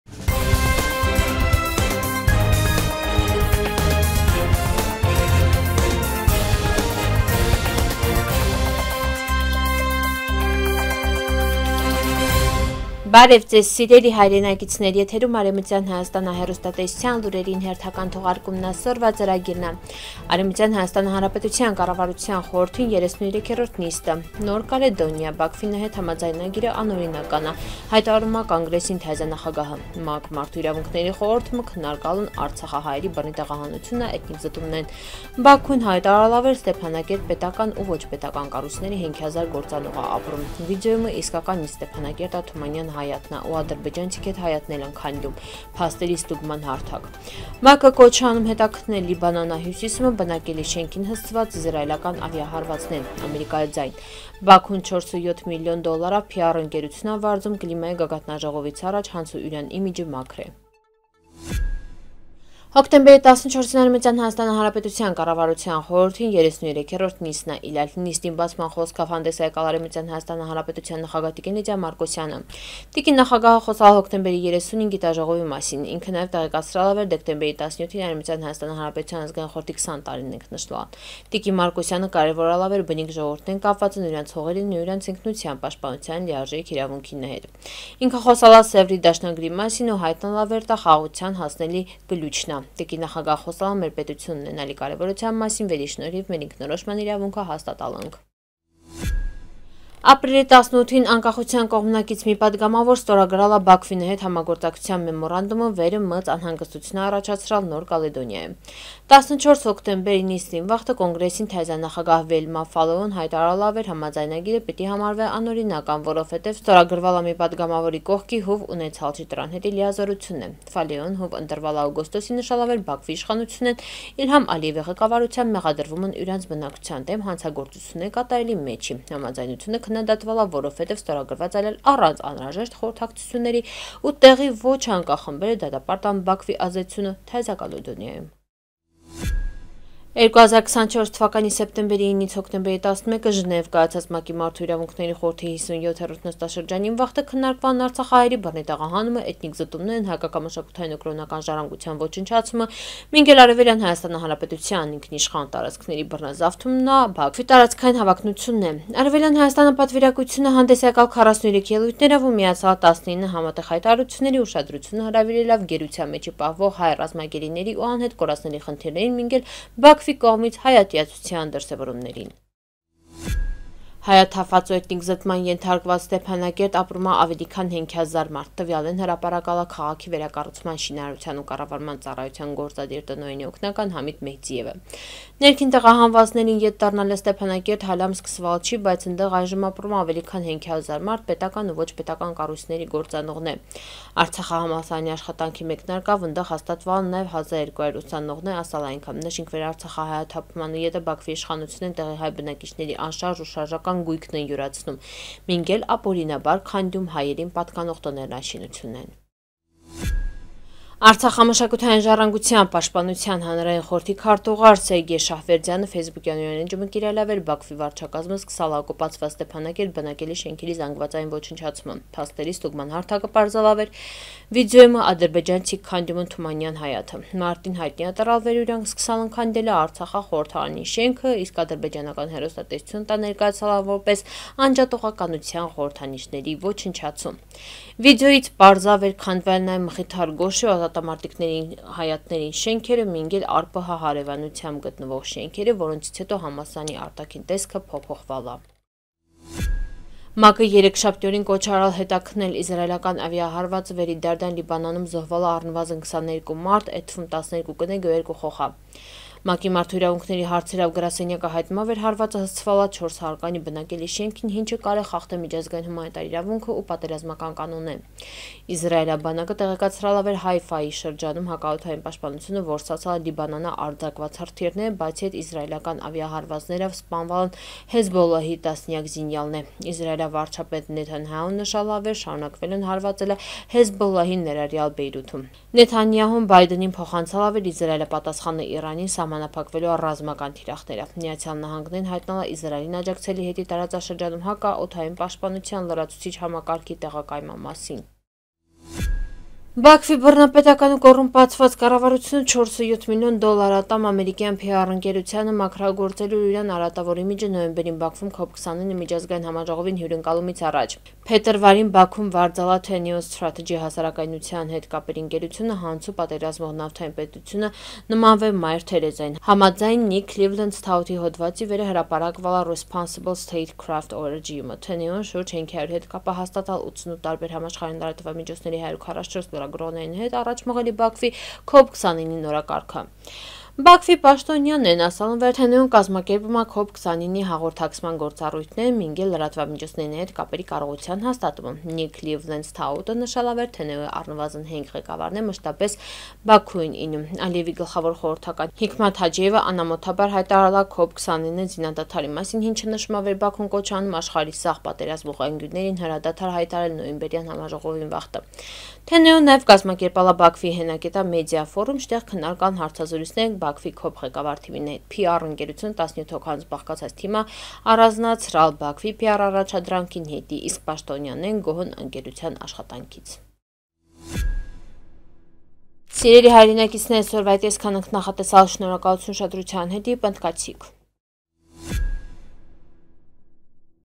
Նոր Կալեդոնիա. Բաքվին նհետ համաձայնագիրը «անօրինական» ա. հայտարարում ա Կոնգրեսին թազա նախագահը
1. ԱՀՀ կառավարության խորհրդի 33֊րդ նիստ
2. Նոր Կալեդոնիա. Բաքվին նհետ համաձայնագիրը «անօրինական» ա. հայտարարում ա Կոնգրեսին թազա նախագահը
3. ՄԱԿ Մարդու Իրավունքների Խորհրդումը քննարկալնԱրցախա Հայերի Բռնի Տեղահանությունն ու Էթնիկ Զտումները
4. Բաքուն հայտարարալա, վեր Ստեփակերտ պետական ու ոչ պետական կառույցների 5000 գործ անողա ապրում
5. Վիդեոումը իսկականից  Ստեփանակերտա Թումանյան հայաթնա  ու  ադրբեջանցիք էդ հայաթնելն քանդում. Փաստերի ստուգման հարթակ
6. ՄԱԿ-ը կոչա անում հետաքննել Լիբանանա հյուսիսումը` բնակելի շենքին հասցված իսրայելական ավիահարվածը. «Ամերիկայի ձայն»
7. Բաքուն 4,7 միլիոն դոլարավ PR ընկերությունա վարձում՝ կլիմայի գագաթնաժողովից առաջ հանցու  ուրան  իմիջը մաքրե